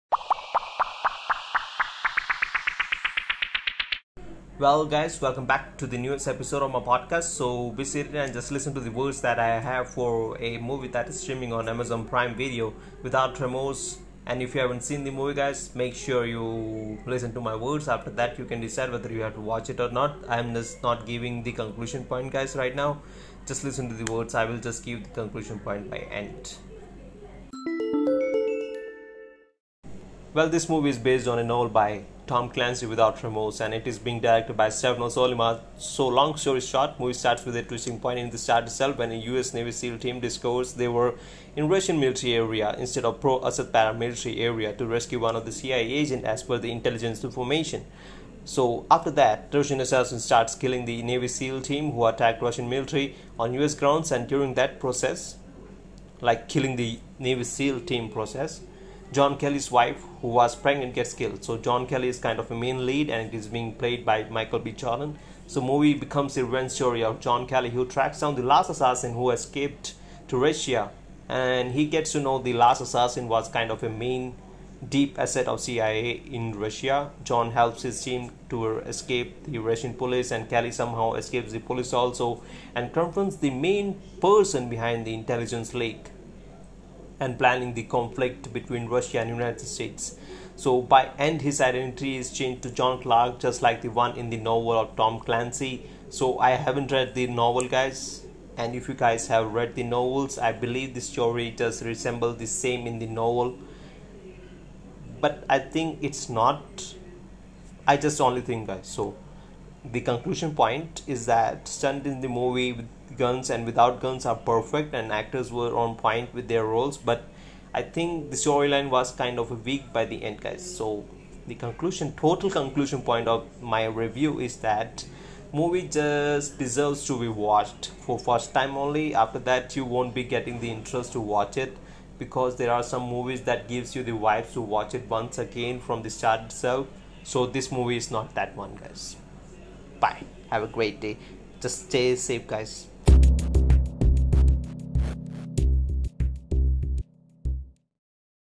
Without Remorse : John Kelly's Revenge story | Streaming
A Long story short thought on Tom Clancy's Without Remorse, streaming on Youtube!! Micheal B Jordan playing as John Kelly later on gets a new name John Clark and why is that?